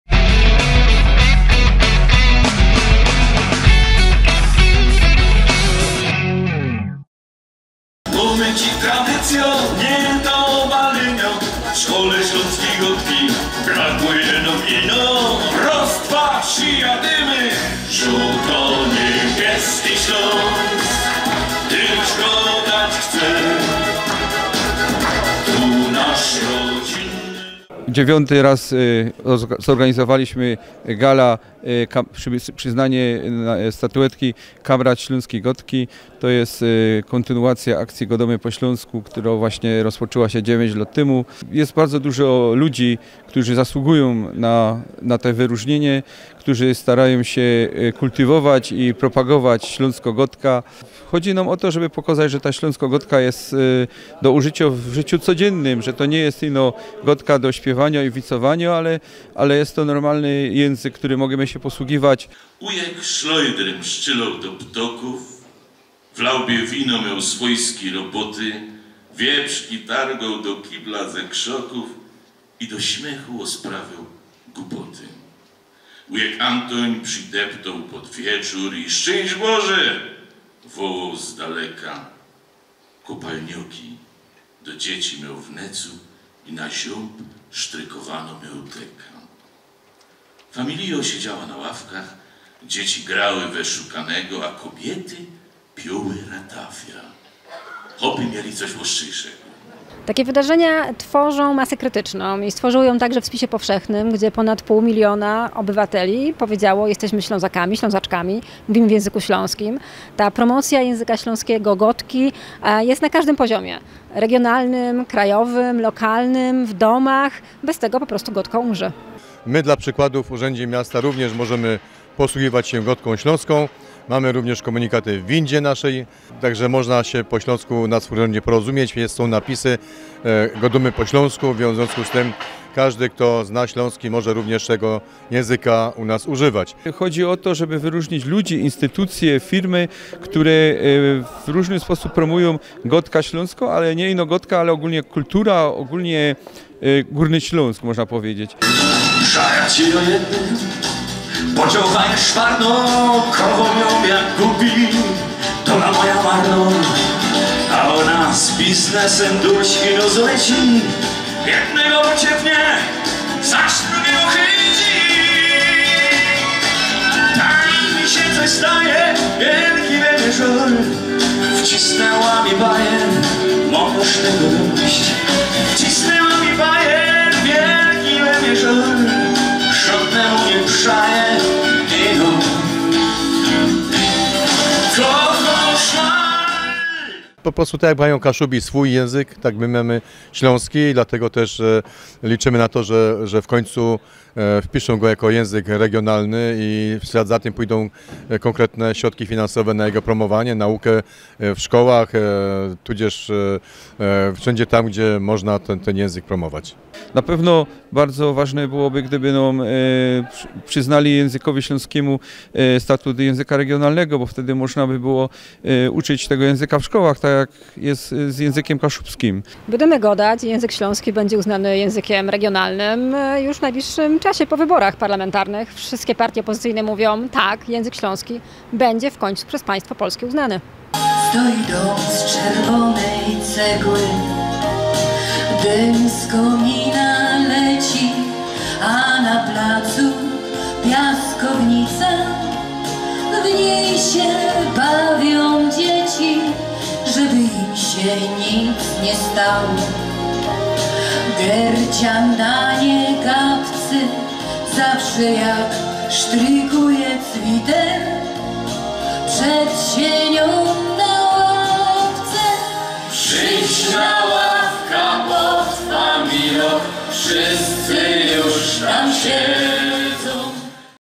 Gala "Godomy po Ślonsku" w Chorzowie
We wtorek 10 października podczas uroczystej gali w Chorzowskim Centrum Kultury zostały wręczone statuetki "KAMRATA ŚLONSKIJ GODKI".